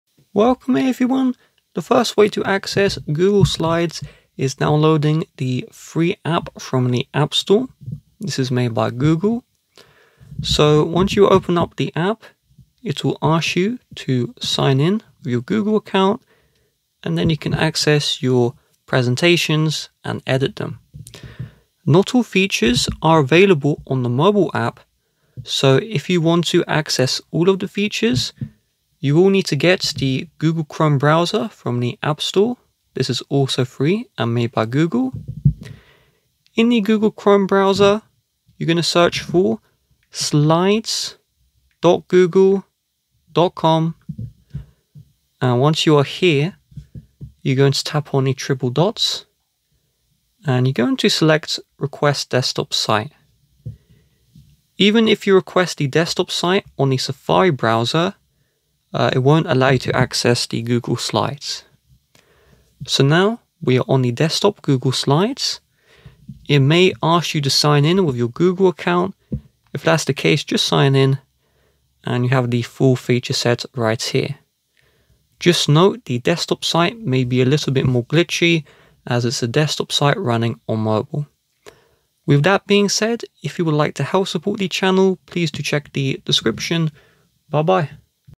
ANY iPhone How To Access Google Slides! (Mobile/Desktop version)
Learn how to access Google Slides on iPhone, both the mobile app, and the desktop site.